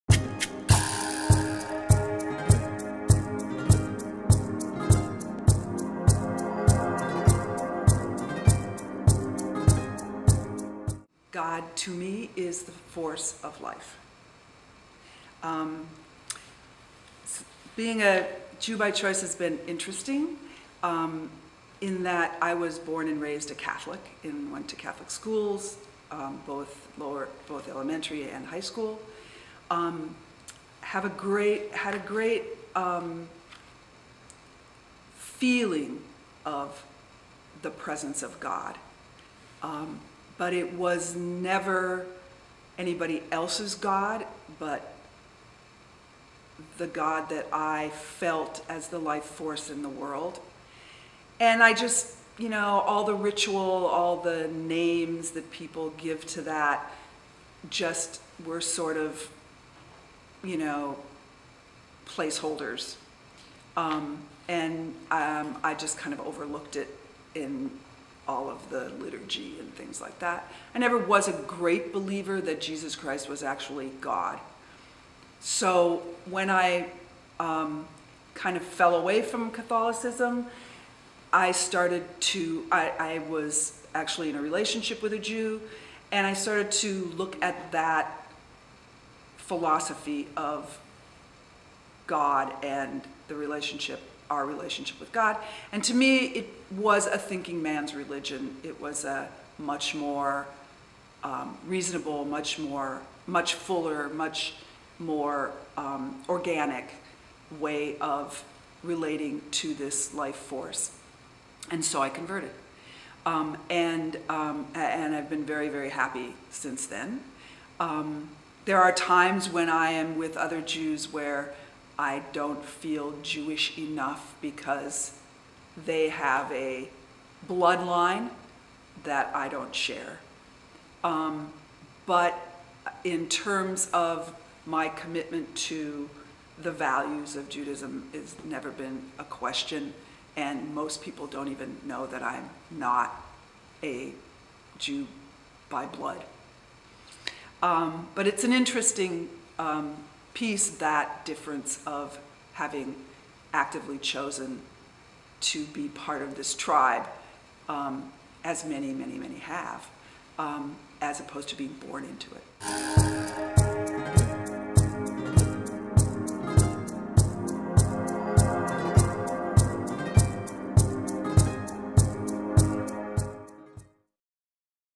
Margaret Hahn on "What is G-d?" part 1
Margaret, Chief Operation Officer, Washington DC JCC, talks about how she came to realize her personal relationship with G-d didn't fit her Catholic upbringing and how she is a Jew By Choice. How she feels Judaism is a much fuller, organic and reasonable relationship with G-d than what she had experienced before.